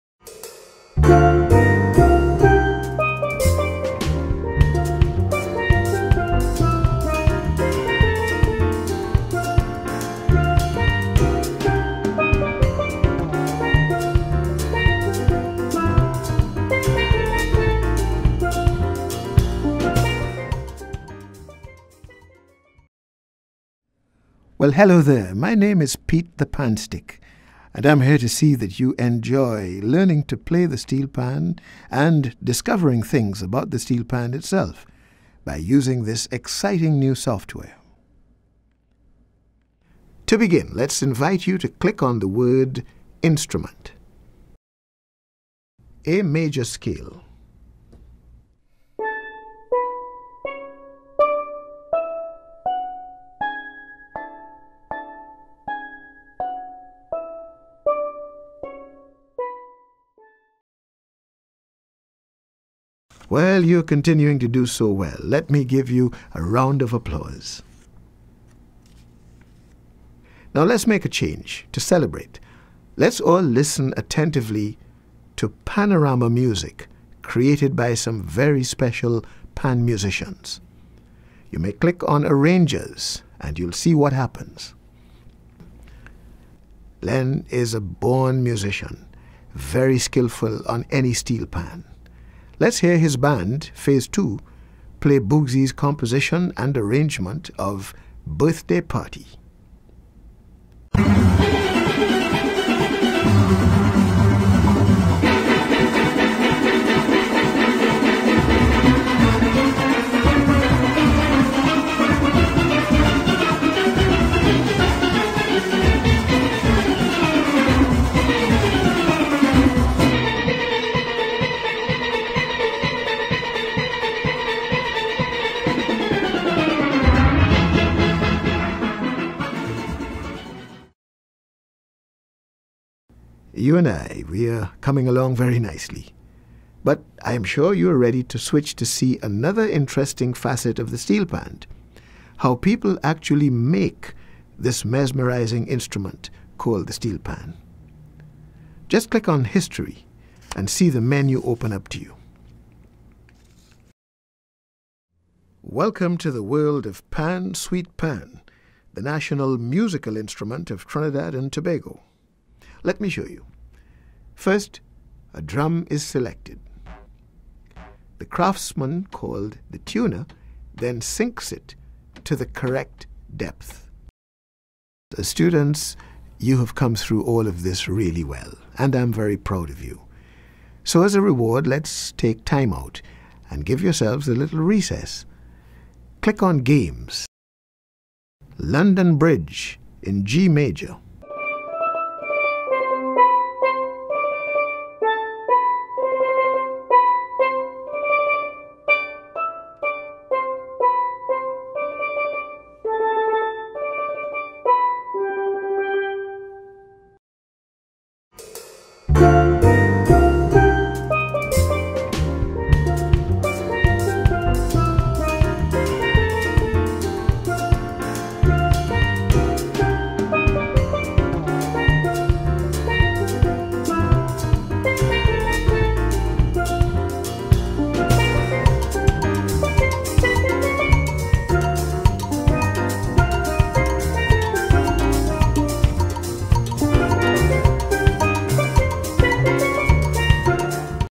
Pete the Panstick Infomercial
An infomercial on the virtual steelpan tutoring software, Pete the Panstick.